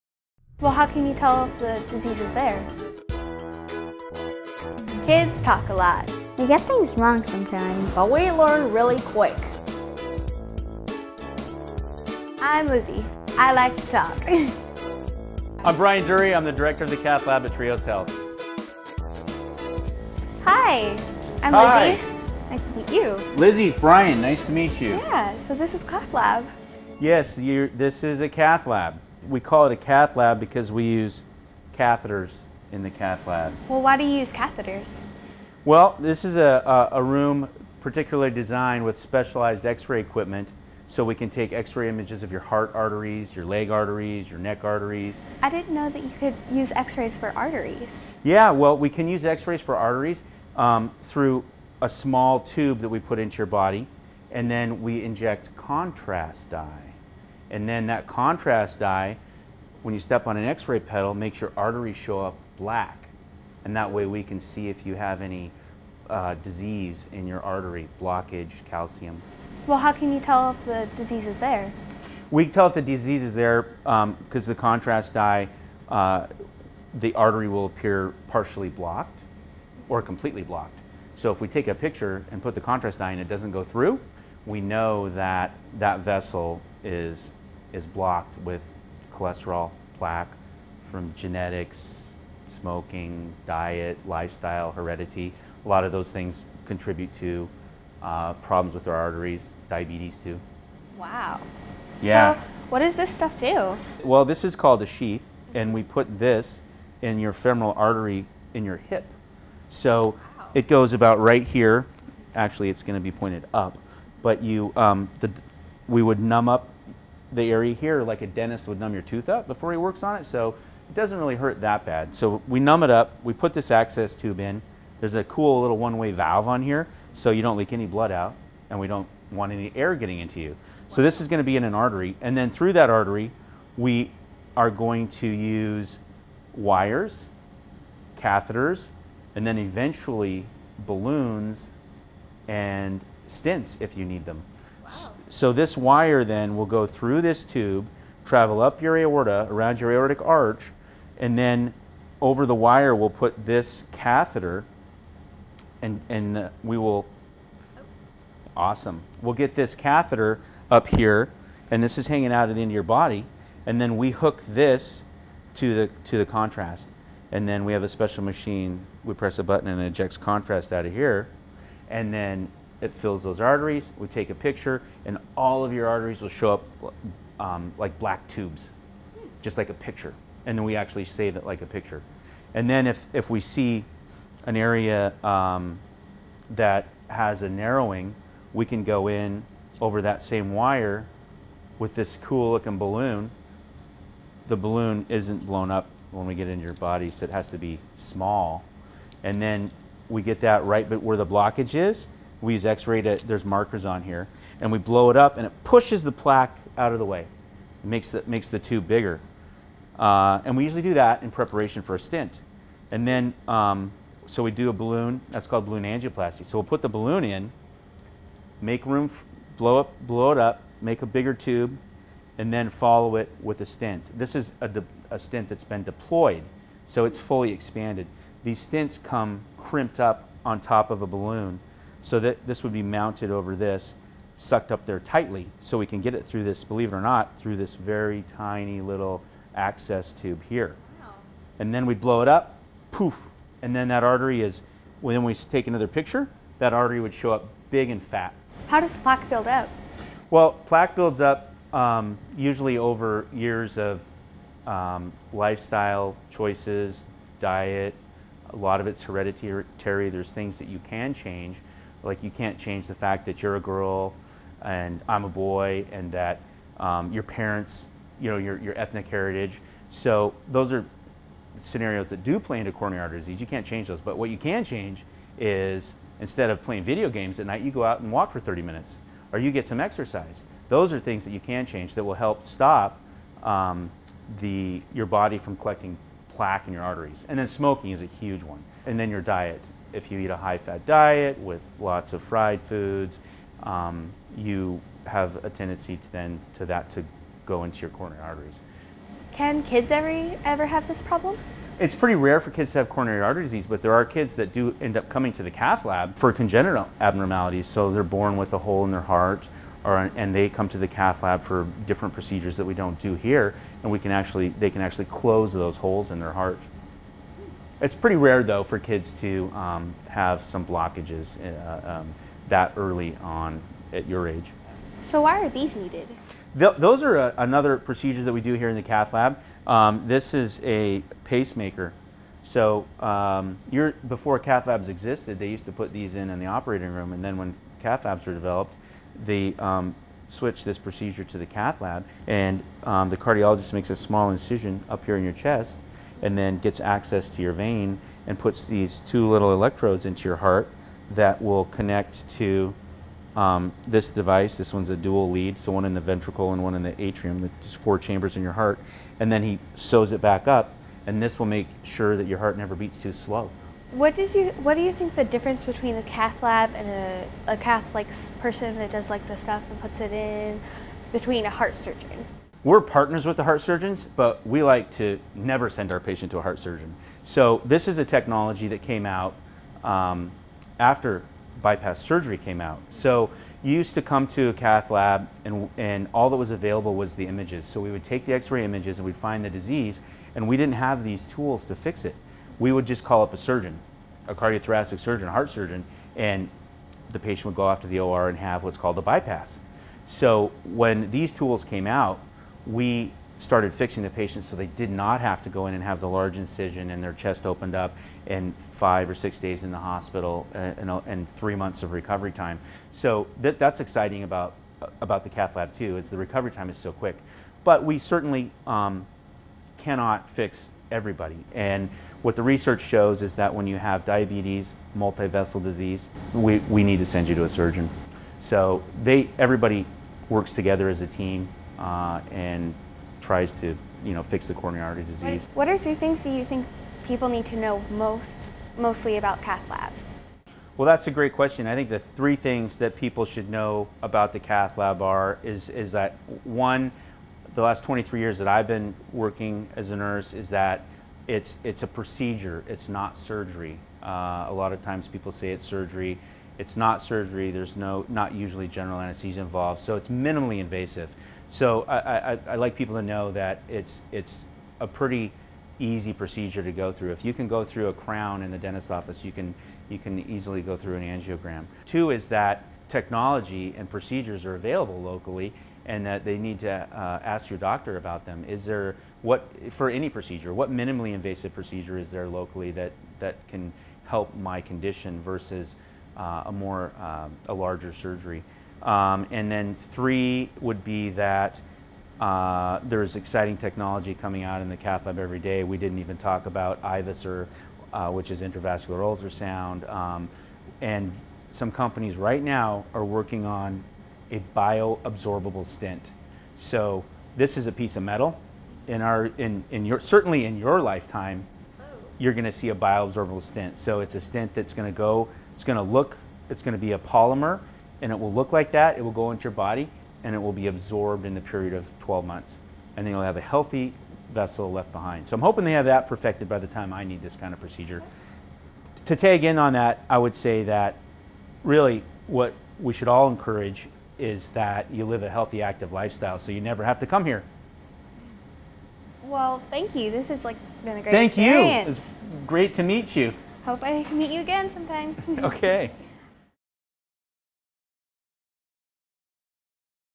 What goes on in a Cath Lab?
Many of us have heard of a Cath Lab. But unless we've had a need for one most of us don't really know what it is and what it's for.

Spokeskid Lizzy dropped in on Brian Dury, director of cardiac services, to check it out and get her questions answered. Take a few minutes to learn the important basics from a kid's perspective. While not as commonly used as a regular doctor's office, it's important that we have these life saving services close to home. We happy to offer two brand-new, state-of-the-art cath labs right here at the new Trios Southridge Hospital.

This content is appropriate for children 8 and older.